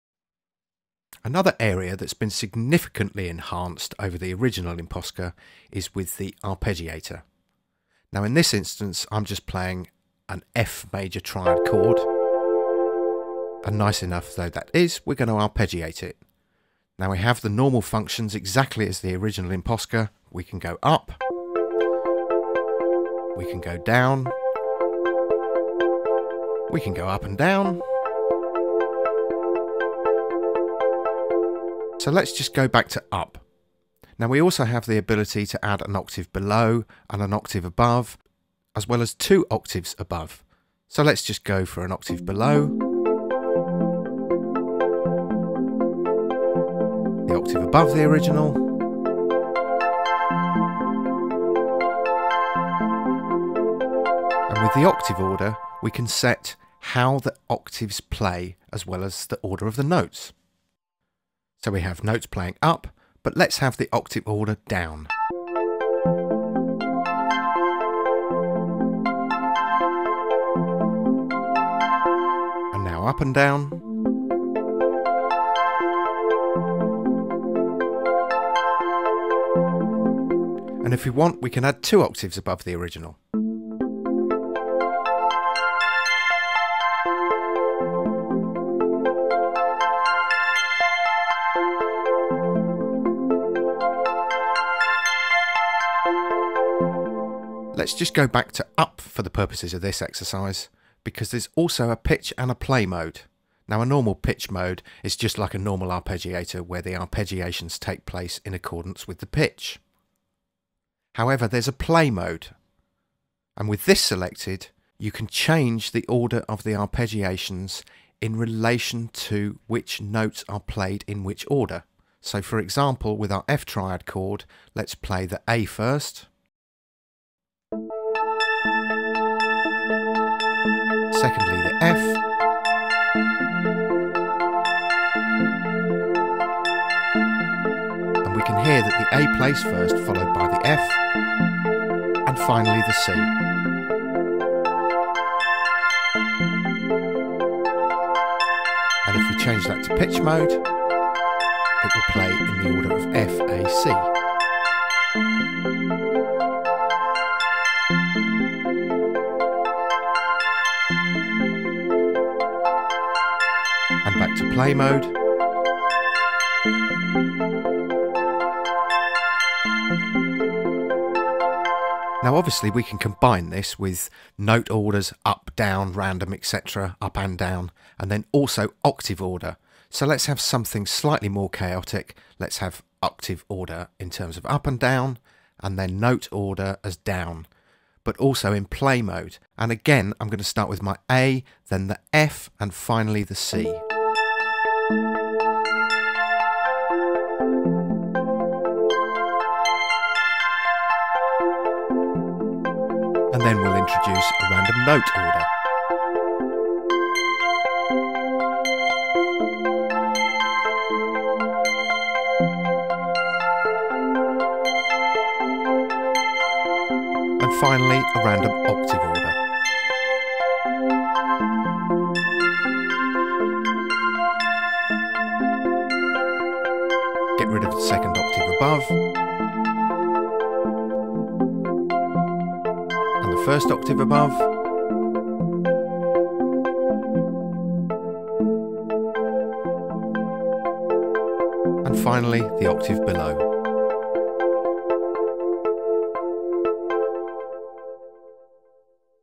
impOSCar2 Tutorial - Arpeggiator
A tutorial showing the updated and improved arpeggiator in impOSCar2